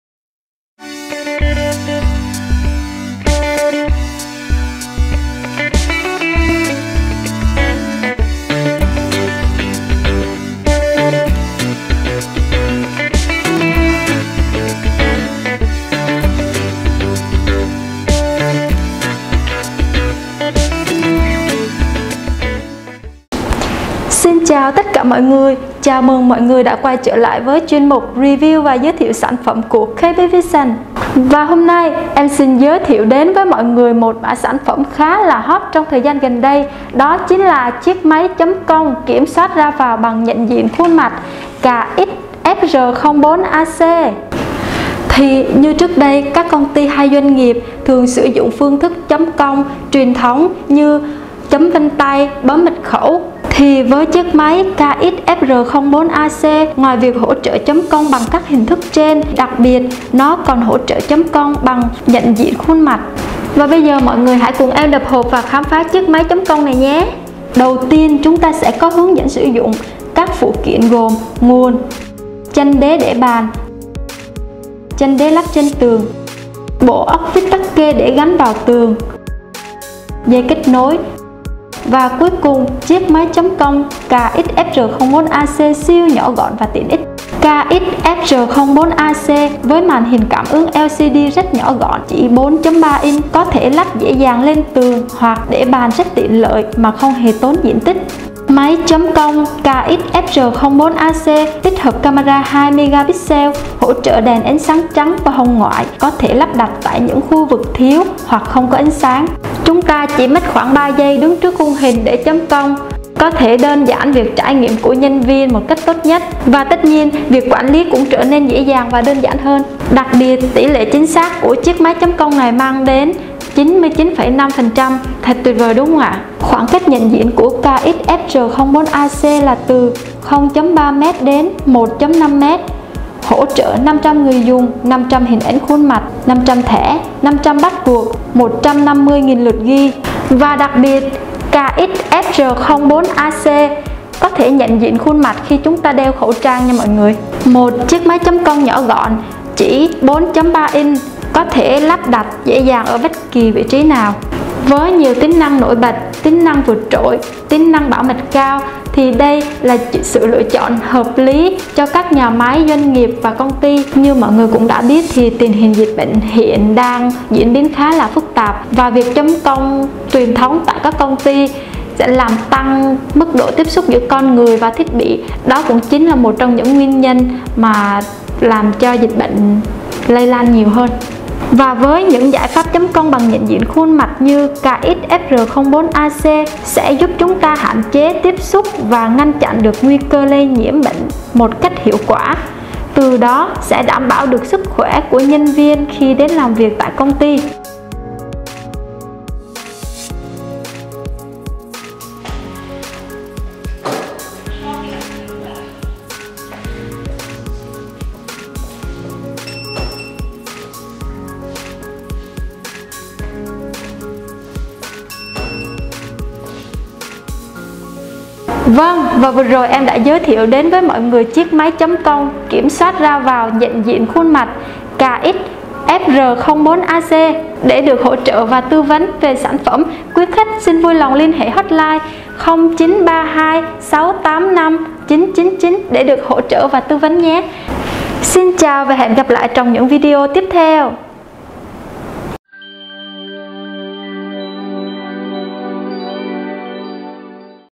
Giới thiệu máy chấm công nhận diện khuôn mặt KX-FR04AC
THIẾT BỊ CHẤM CÔNG - KIỂM SOÁT RA VÀO NHẬN DIỆN KHUÔN MẶT
. Màn hình cảm ứng LCD 4.3inch
. Tích hợp camera 2.0MP, hỗ trợ đèn ánh sáng trắng và hồng ngoại.
. Chấm công, kiểm soát ra vào bằng khuôn mặt, thẻ từ và mật khẩu.
. Hỗ trợ 500 người dùng, 500 hình ảnh khuôn mặt, 500 thẻ, 500 password,150.000 lượt ghi.
. Có thể hoạt động độc lập và offline.
. Khoảng cách nhận diện từ 0.3m – 1.5m
. Tốc độ nhận diện 0.3s/ người, tỉ lệ chính xác 99.5%.
. Hỗ trợ phát hiện khuôn mặt thật hoặc ảnh.
. Nguồn DC12v, 1.5A
. Lắp trong nhà (trên tường hoặc để bàn), thiết kế nhỏ gọn, hiện đại